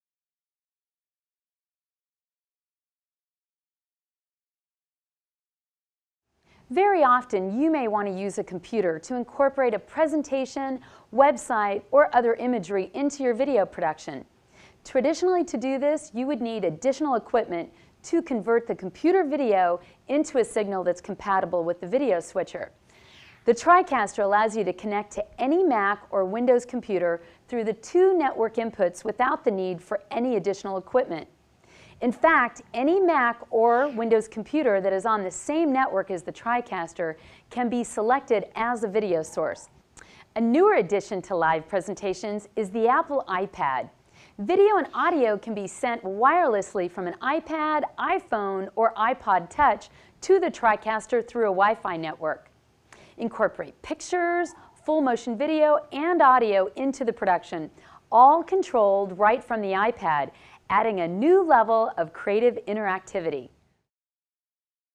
TriCaster 455 - Network Inputs
Add digital media from four built-in players and 2 network sources that share video and audio from any Windows PC, Mac or wireless iOS device.

Copyright NewTek .Inc